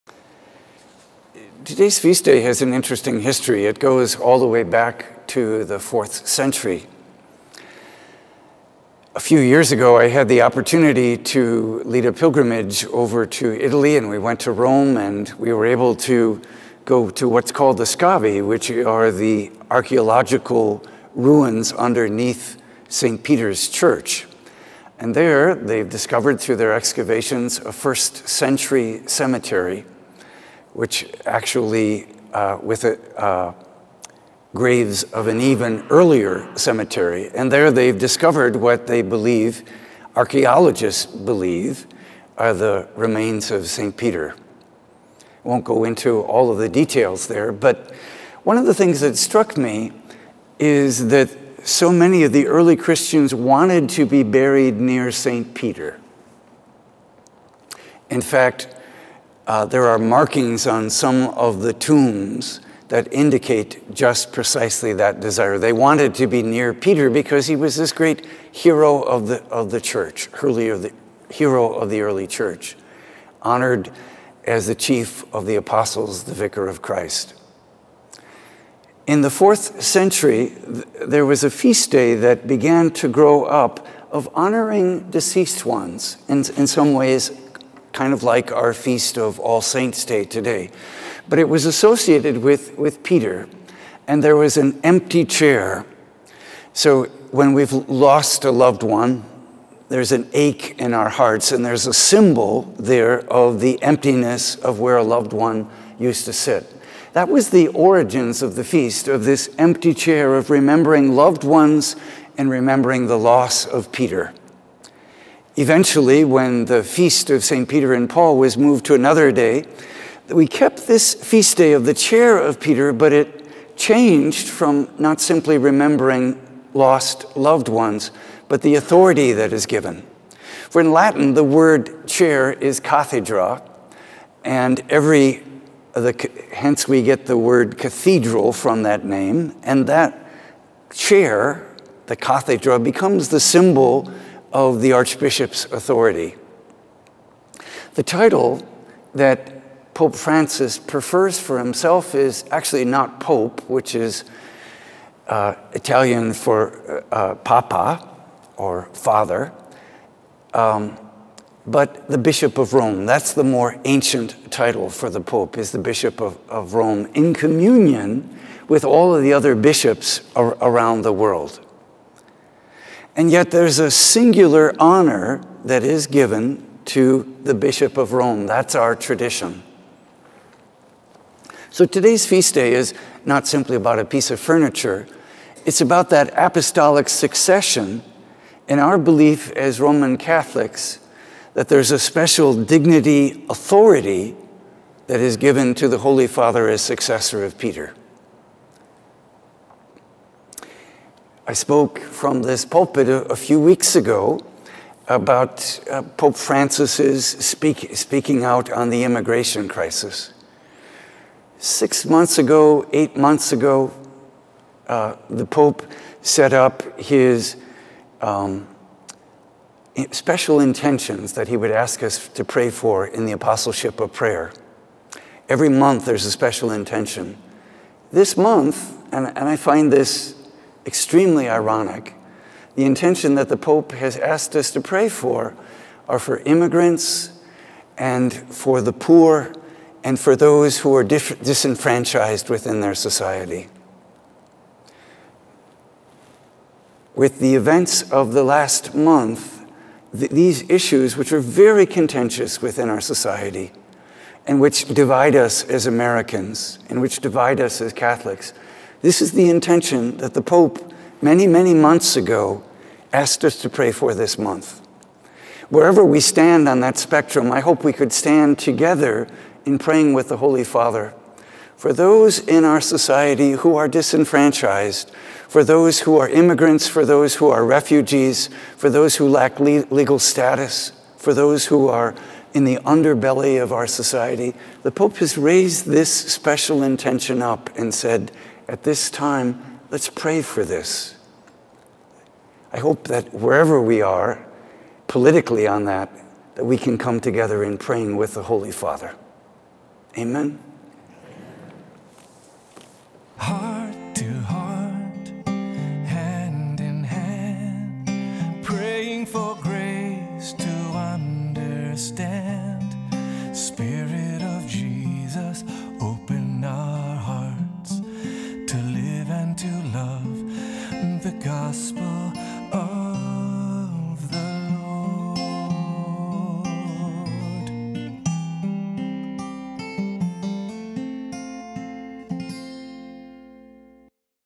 Video Homily: The Chair of St Peter - Short Version
Can you find St. Peter's tomb? Watch today's homily with Fr. Michael to learn about the ancient burial ground beneath the Vatican said to be St. Peter's final resting place. Learn about Peter's successors, the Popes, and the special prayer intentions offered this month by Pope Francis.